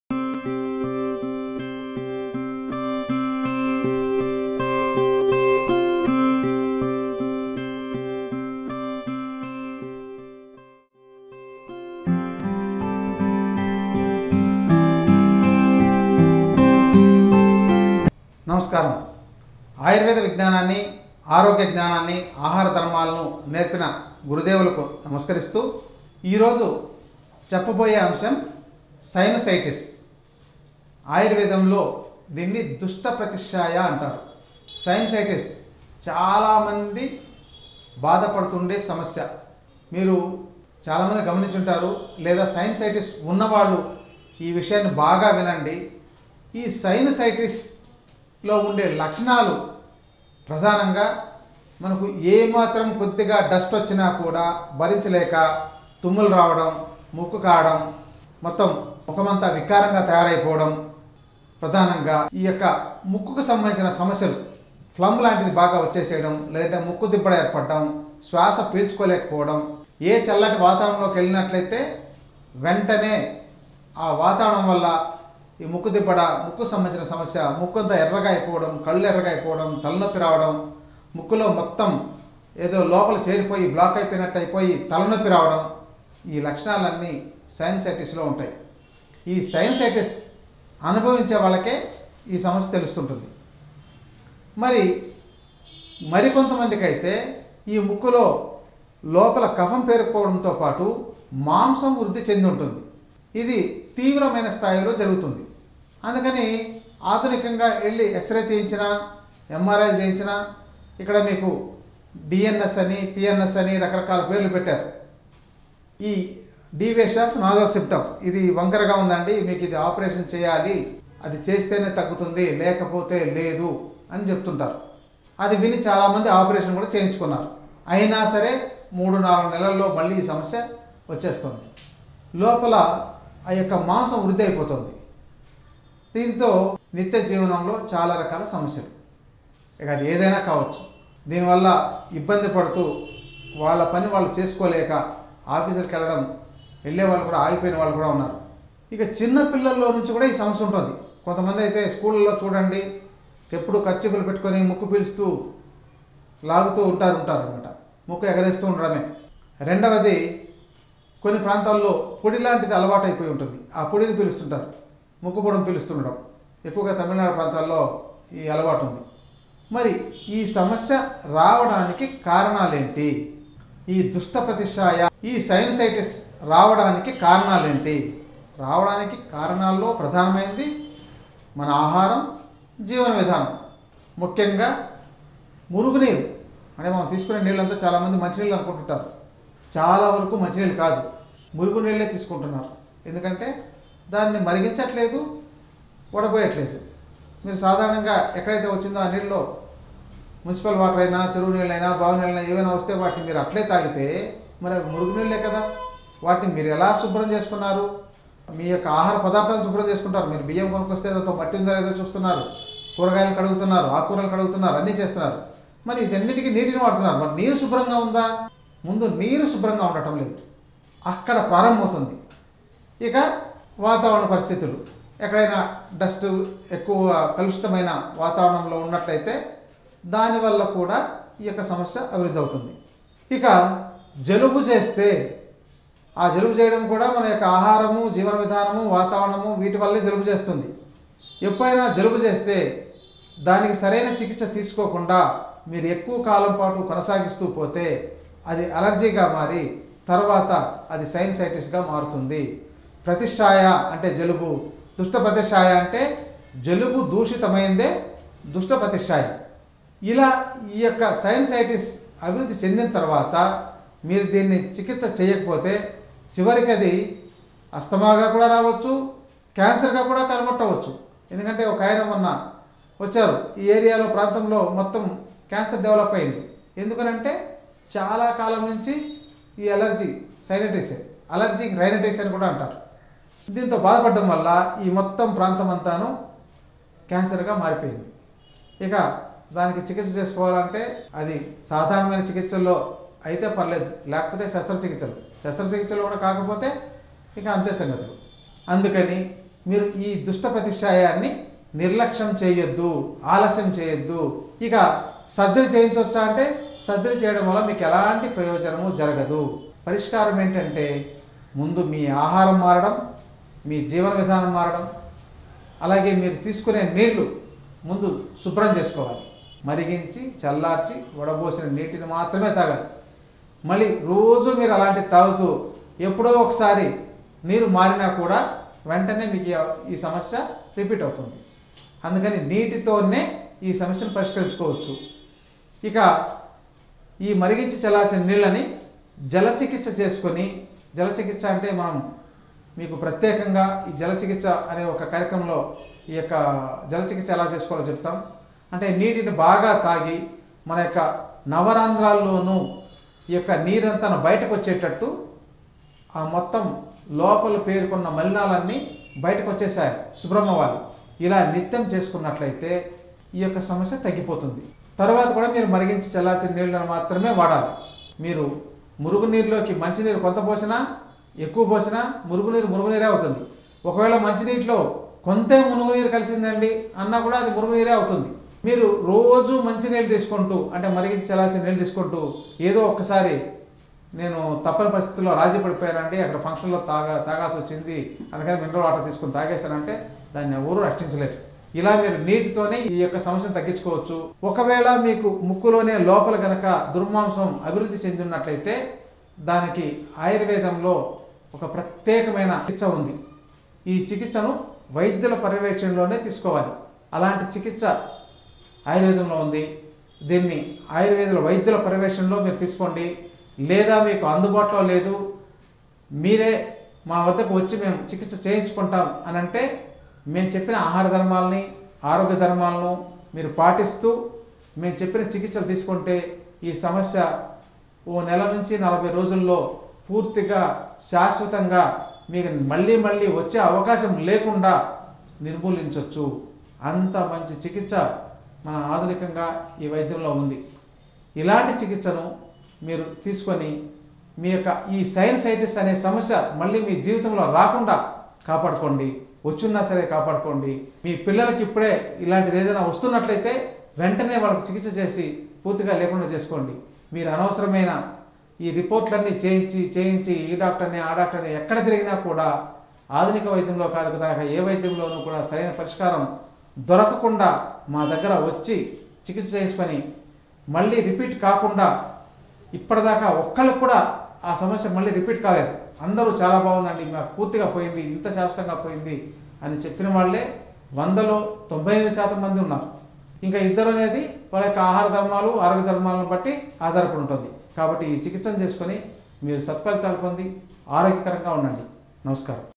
Permanent Treatment for Sinusitis in Ayurveda by Dr. N. Subrahmanyam
Amrutham Ayurveda Hospital
Dilsukhnagar, Hyderabad - 500 060.